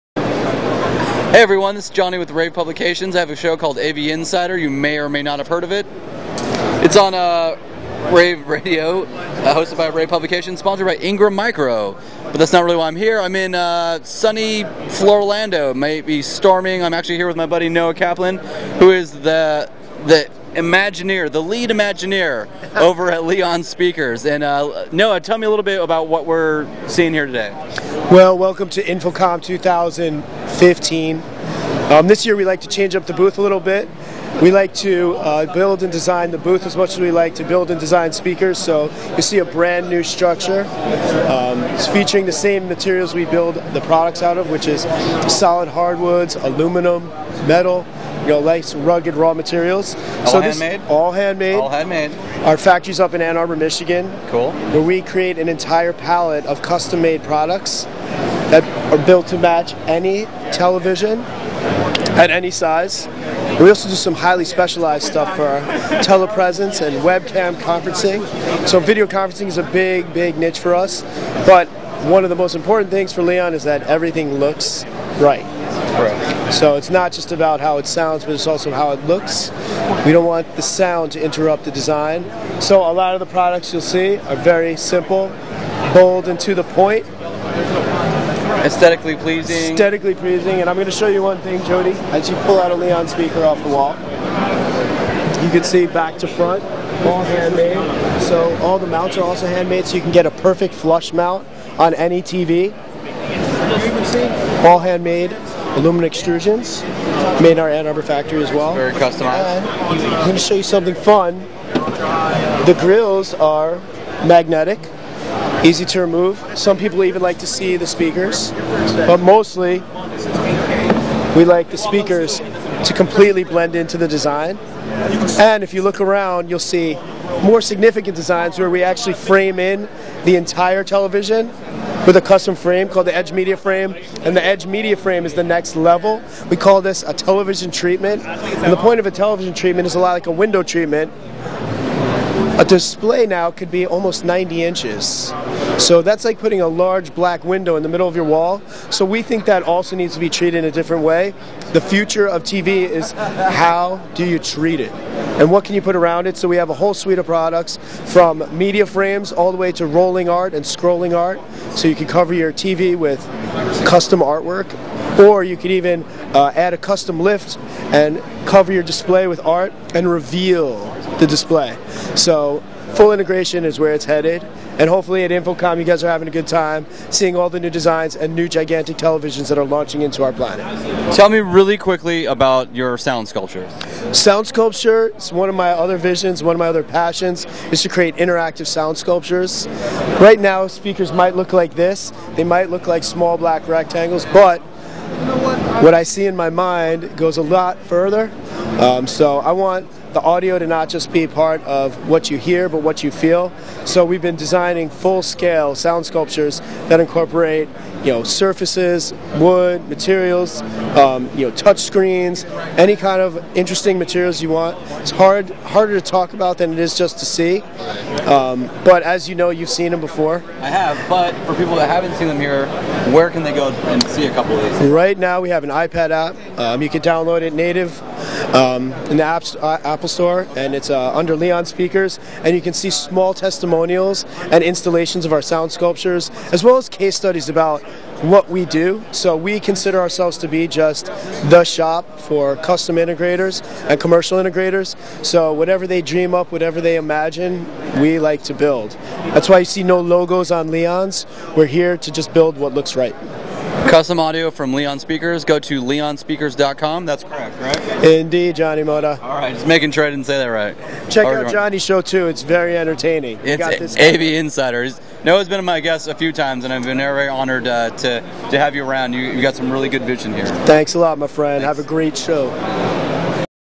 InfoComm 2015: Johnny Mota Speaks With Leon Speakers' Noah Kaplan About Their Custom Audio Solution
InfoComm 2015: Johnny Mota Speaks With Noah Kaplan, President of Leon Speakers, About Their Custom Audio Solutions From Commercial Audio Products to High End Residential Products